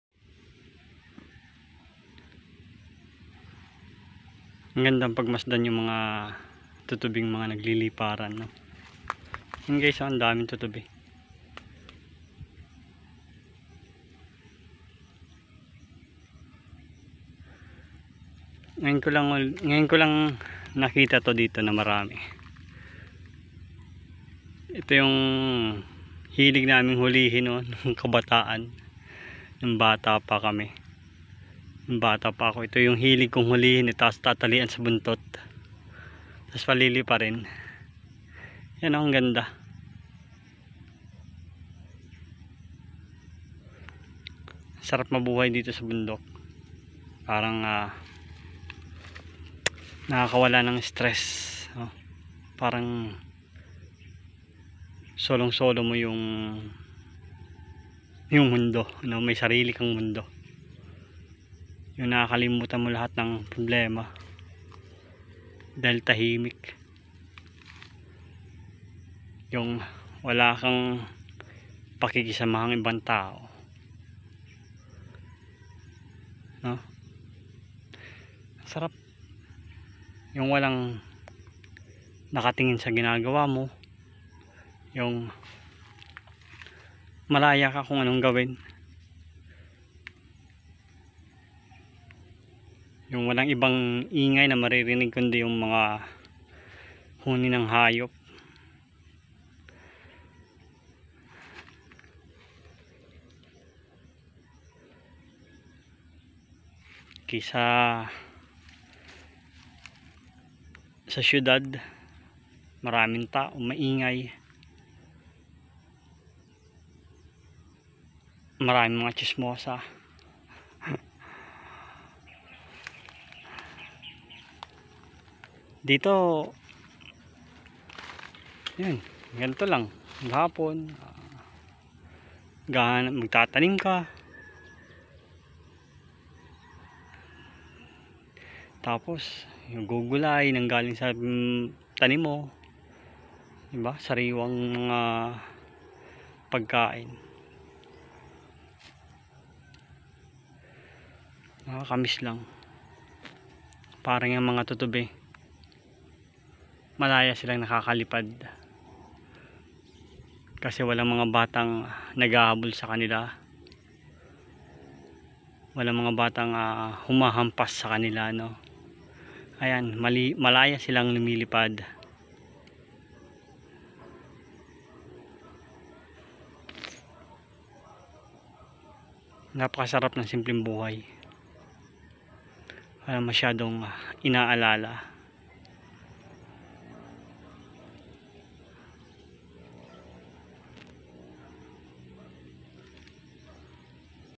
SIMPLENG BUHAY LANG SAPAT NA
Masarap tumira sa bukid lalo na kung gusto mong takasan ang ingay ng mga tao.